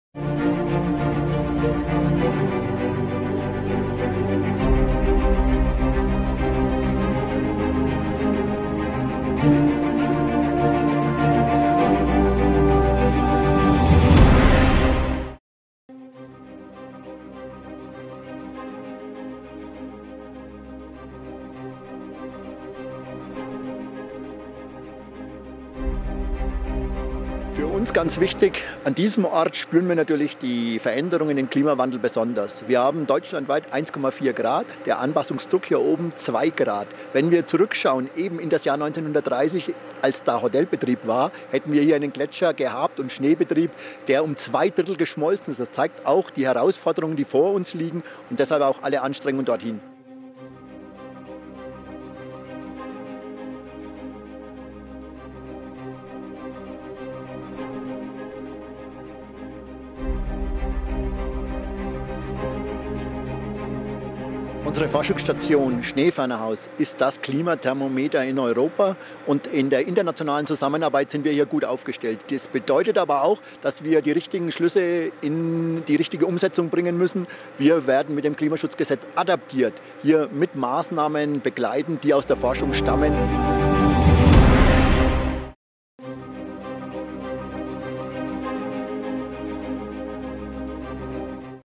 20 Jahre Umweltforschungsstation Schneefernerhaus
Unabhängige Klimaforschung ist elementar wichtig. Wir müssen den Klimawandel verstehen, um die richtigen Maßnahmen zu ergreifen. Die Umweltforschungsstation Schneefernerhaus spielt dabei eine besondere Rolle. Das Schneefernerhaus soll zu einem internationalen Klimathermometer ausgebaut werden. Denn in den Alpen sieht man den Klimawandel wie unter einem Brennglas. Die Alpen sind damit ein Micro-Labor zur Erforschung des Klimawandels und seiner Auswirkungen auf Mensch und Natur. Dank modernster Labore und engagierter Wissenschaftler werden auf der Zugspitze vielfältige Informationen über das Klima gewonnen.